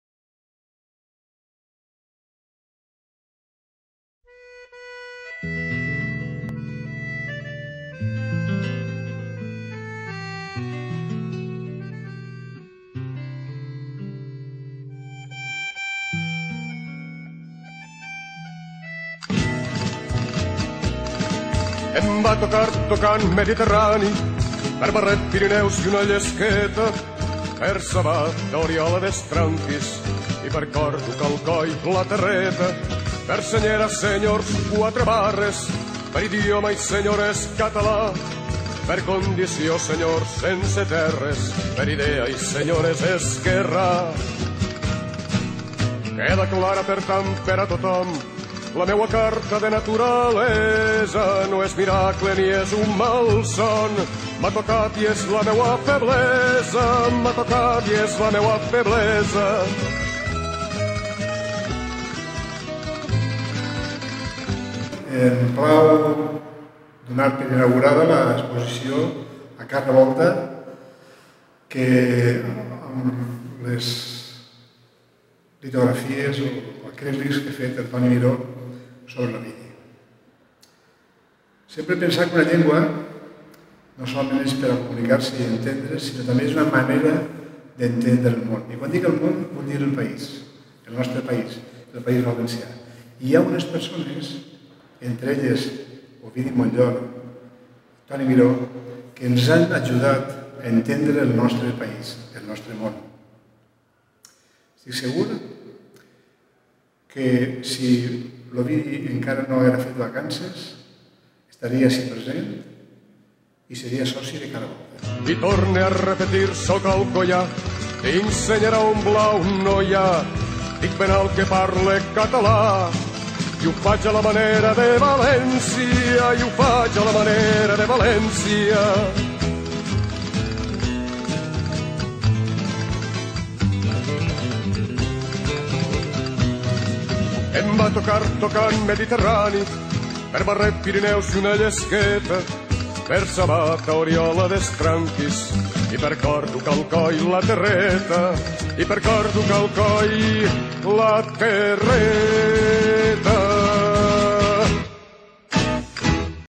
L`Olvidi popular
Obres d’Antoni Miró sobre la figura d’Ovidi Montllor.
Vídeo: Julián Affranchino